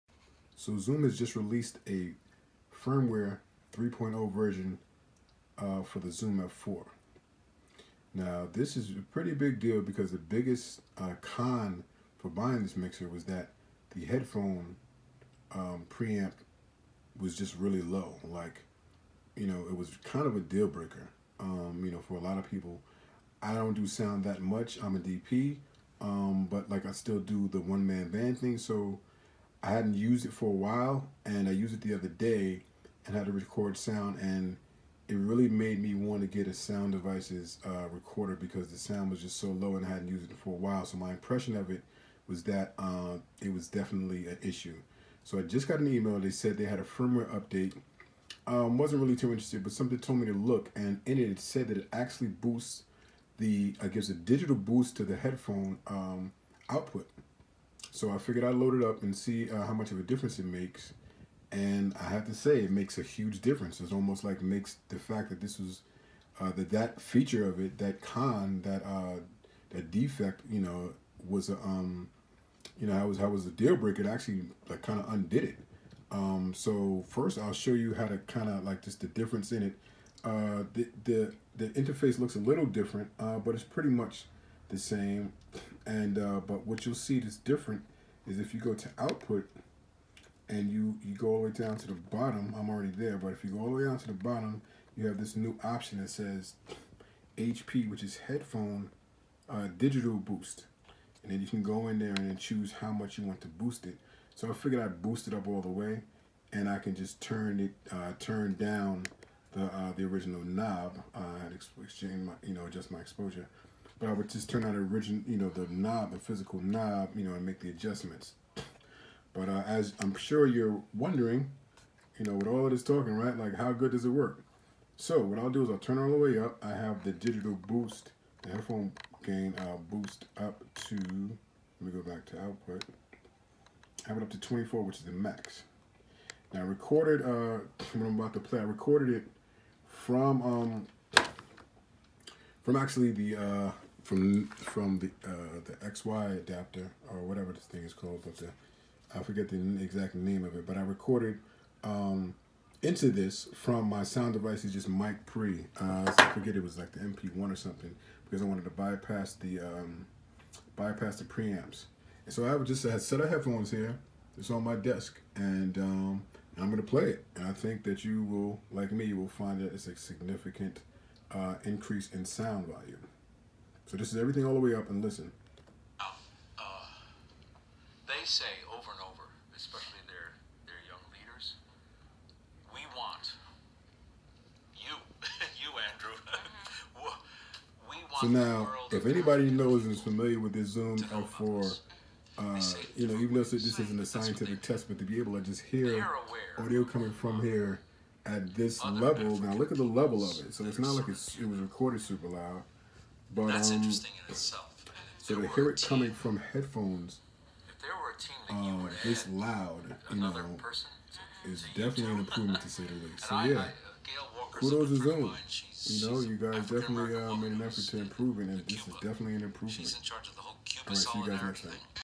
ZOOM F4 Low Headphone Volume FIXED ***
The well known problem & deal breaker for many as far as using the Zoom F4 is officially fixed!

The fix came in the latest firmware update as well as some other improvements & bug fixes. I think it's a fix, let me know what you think.